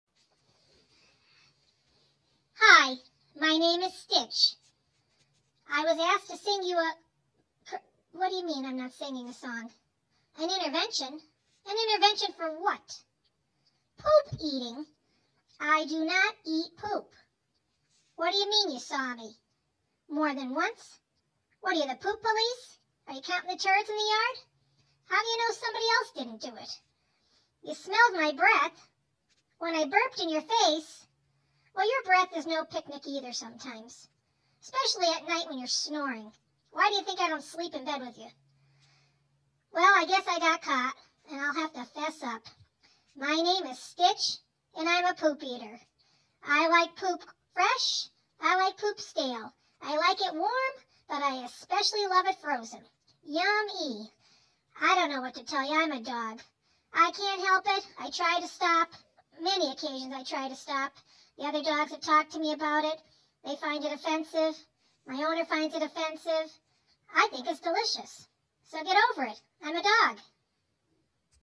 Stitch was tricked! The intervention.
Stich has to confront her poop eating!